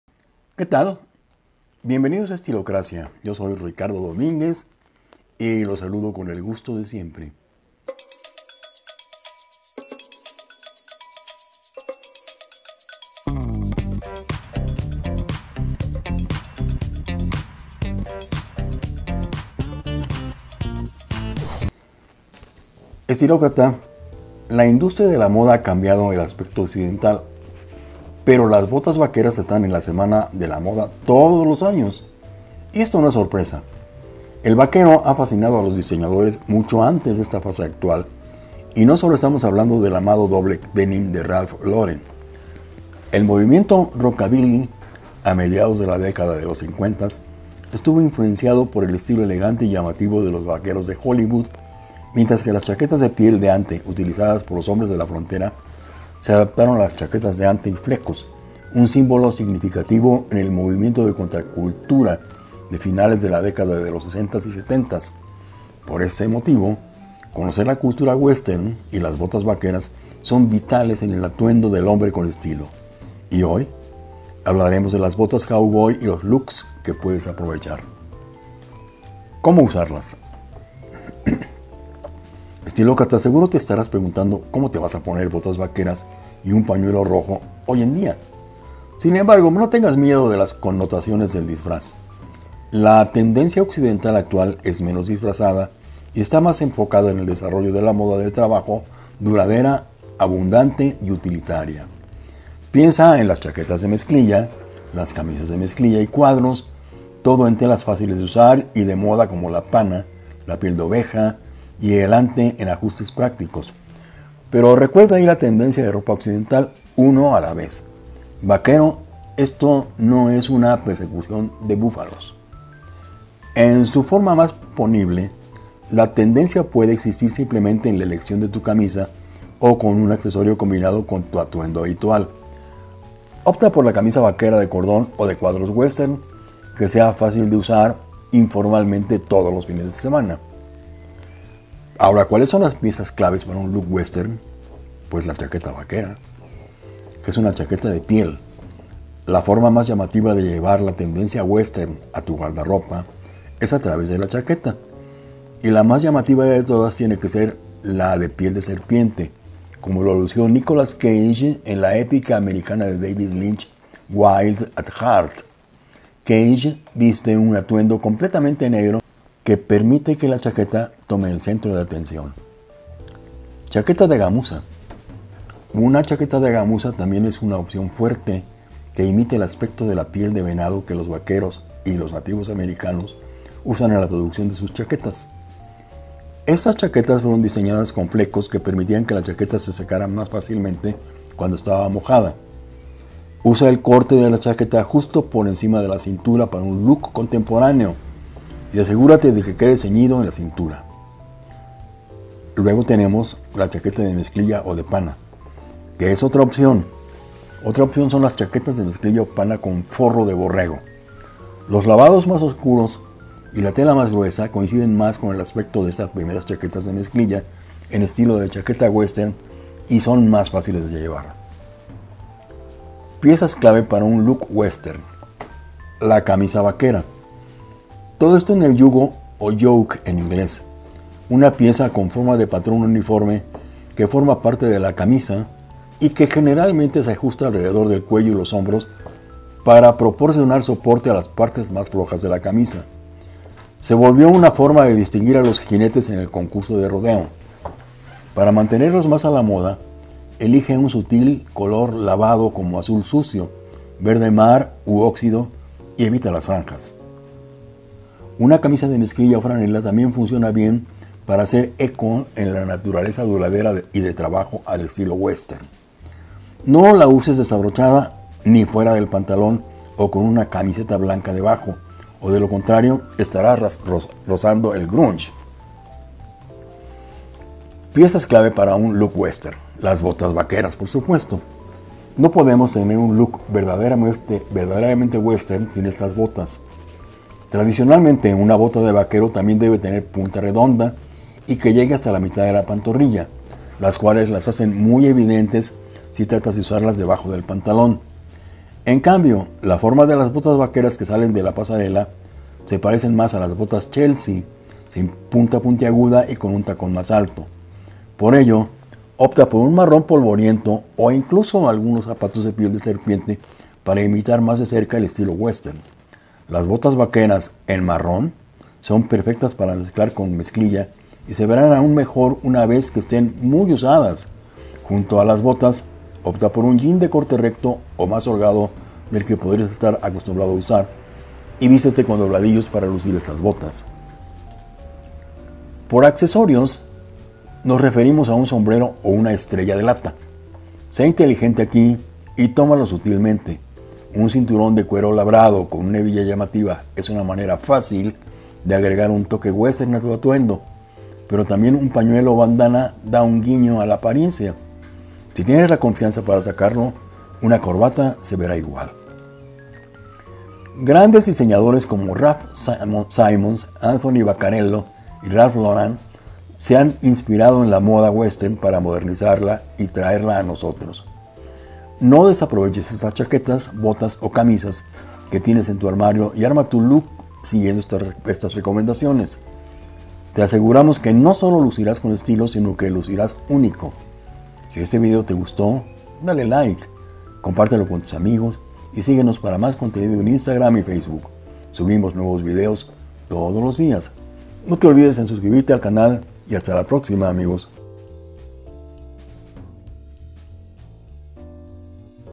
BOTAS VAQUERAS combínalas para lograr UN LOOK WESTERN DE HOMBRE
LAS BOTAS VAQUERAS SIEMPRE ESTARÁN DE MODA, Y TE HARÁN LUCIR MASCULINO. APRENDE A LUCIRLAS DE LA MEJOR MANERA

00:00 Introducción
01:15 Titulo Look botas western
01:39 Chamarra
03:52 Camisa
04:46 Botas
05:35 Accesorios
06:25 Despedida

Visita nuestro sitio asociado de Amazon para productos de tu interés: